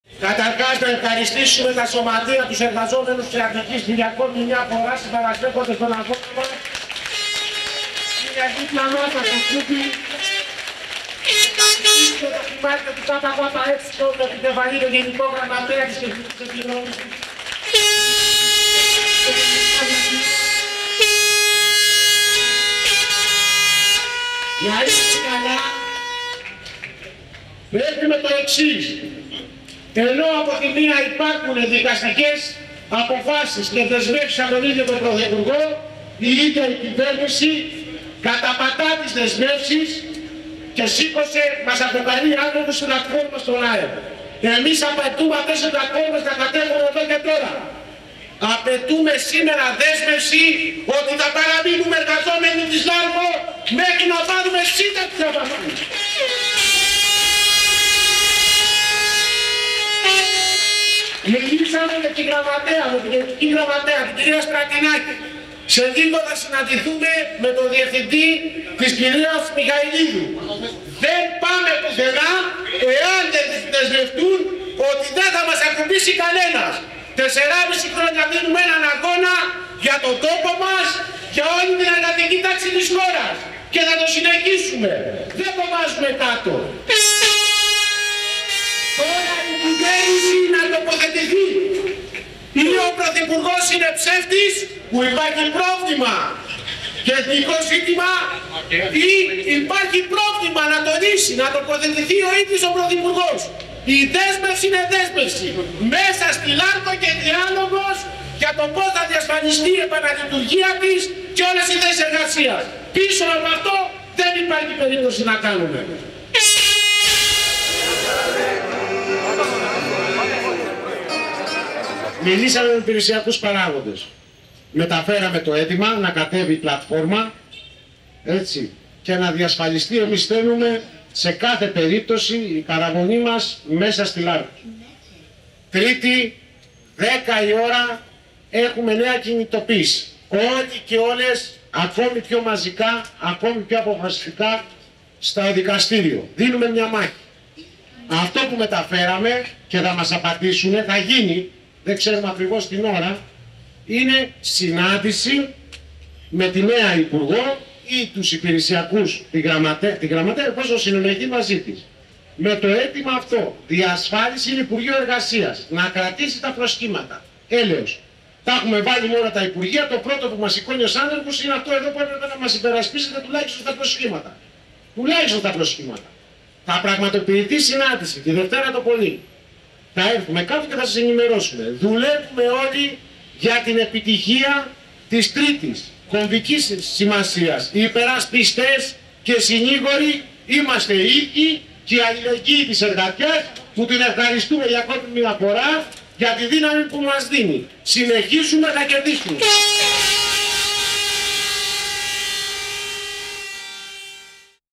Π. Πολίτης: Συνεχίζουμε - Όλοι την Τρίτη στην Ευελπίδων
Δυναμική κινητοποίηση στο υπουργείο Εργασίας πραγματοποίησαν οι εργαζόμενοι της ΛΑΡΚΟ και τα Σωματεία τους.

Αντιπροσωπεία των Σωματείων συναντήθηκε με υπηρεσιακούς παράγοντες του υπουργείου Εργασίας στους οποίους μετέφεραν την απαίτηση τους να κατέβει άμεσα η πλατφόρμα των προγραμμάτων ανεργίας που τους χαρακτηρίζει άνεργους απολυμένους και ανανεωθούν οι συμβάσεις εργασίας τους μέχρι να καπνίσει ξανά η τσιμινιέρα της ΛΑΡΚΟ.

Ο αγώνας, η άμεση αντίδραση και η αποφασιστικότητα των εργατών της ΛΑΡΚΟ στρίμωξε ξανά την κυβέρνηση. Απέσπασαν τη δέσμευση ότι τη Δευτέρα 17 Ιούνη θα έχουν συνάντηση με τη νέα υπουργό Εργασίας.

Μετά τη δέσμευση που απέσπασαν, οι εργαζόμενοι της ΛΑΡΚΟ, οι οικογένειές τους και οι συνάδελφοι τους από Στερεά Ελλάδα και Αττική ανανέωσαν το αγωνιστικό τους ραντεβού για την Τρίτη 18 Ιούνη στις 10 π.μ. στα Δικαστήρια Ευελπίδων στην Αθήνα και την εκδίκαση των ασφαλιστικών μέτρων που έχουν καταθέσει τα Σωματεία ενάντια στις απολύσεις τους.

Στην κινητοποίηση παραβρέθηκε πολυμελής αντιπροσωπεία της ΚΕ του ΚΚΕ, με επικεφαλής τον ΓΓ της ΚΕ του ΚΚΕ, Δημήτρη Κουτσούμπα, ενώ επίσης συμμετείχαν τα μέλη του ΠΓ της ΚΕ Γιώργος Μαρίνος, Κώστας Παρασκευάς, Γιάννης Πρωτούλης, Θοδωρής Χιώνης.

«Μεταφέραμε το αίτημα να κατέβει η πλατφόρμα (σ.σ. της ΔΥΠΑ) και να διασφαλιστεί η παραμονή μας μέσα στη ΛΑΡΚΟ» τόνισε ο Παναγιώτης Πολίτης, πρόεδρος του Σωματείου Εργαζομένων της ΛΑΡΚΟ Λάρυμνας μετά τη συνάντηση με υπηρεσιακούς παράγοντες.

«Δίνουμε μια μάχη» επεσήμανε και κάλεσε σε ακόμα πιο μαζική και αποφασιστική συμμετοχή στην κινητοποίηση της Τρίτης στην Ευελπίδων. «Υπερασπιστές και συνήγοροι είμαστε οι ίδιοι και η αλληλεγγύη της εργατιάς που την ευχαριστούμε για ακόμη μια φορά για τη δύναμη που μας δίνει. Συνεχίζουμε. Θα νικήσουμε» τόνισε.